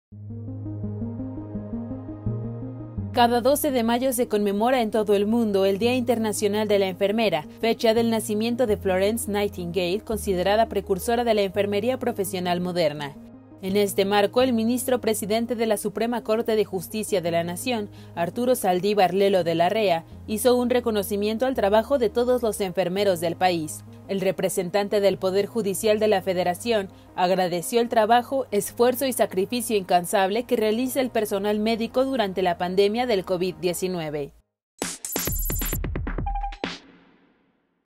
SCJN aplaude labor de enfermeros del país
Cada 12 de mayo se conmemora en todo el mundo el Día Internacional de la Enfermera, fecha del nacimiento de Florence Nightingale, considerada precursora de la enfermería profesional moderna.